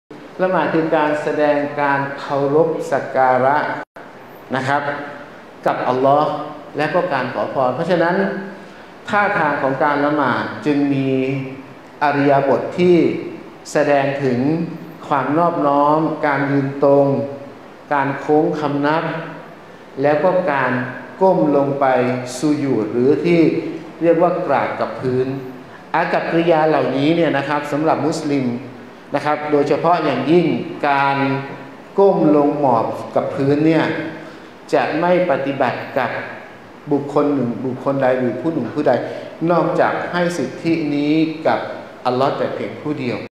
การก้มกราบสงวนไว้สำหรับพระเจ้าเท่านั้น | เรียนรู้อิสลามวันละนิด จิตแจ่มใส
ท่าทางการละหมาดที่มุสลิมใช้ในการเคารพสักการะอัลลอฮฺนั้น
มีแบบอย่างมาจากท่านนบีมุฮัมมัด โดยเฉพาะท่าทางการสูญูด(ก้มกราบ)
มุสลิมจะสงวนไว้สำหรับพระเจ้าเท่านั้น